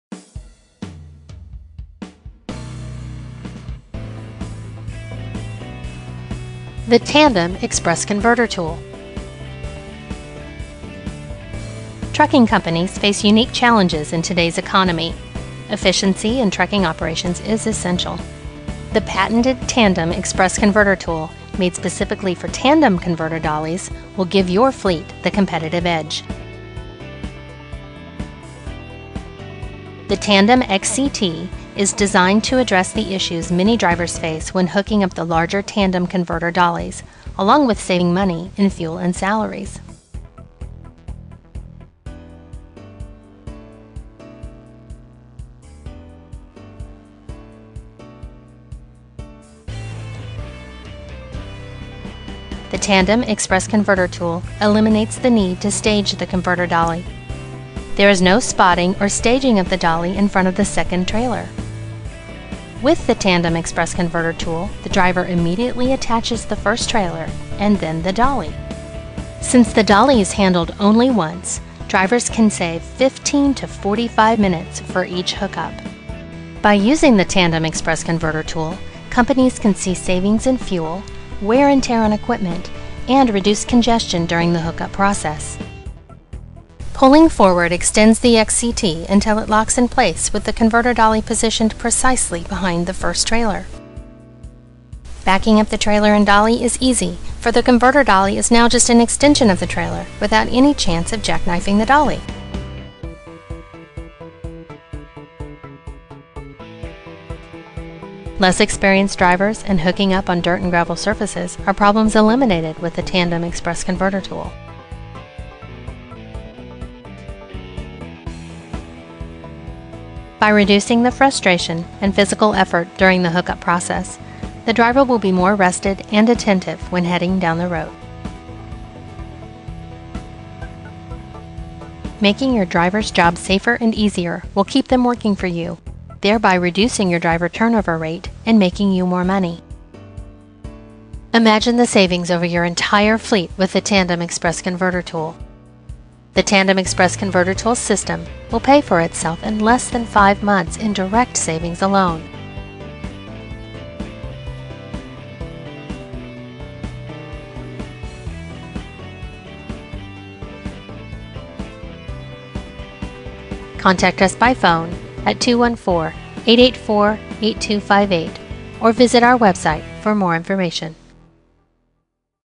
The Tandem X-press Converter Tool! Find out more at Xpressconvertertool.com
Today, drivers face many challenges hooking up the larger tandem converter dollies. Introducing the Tandem X-Press Converter Tool designed for tandem axle dollies! Now, even tandem dollies can connect with ease using the XCT. Watch how quickly and safely this driver connects.  You will be amazed at how easy it is to use!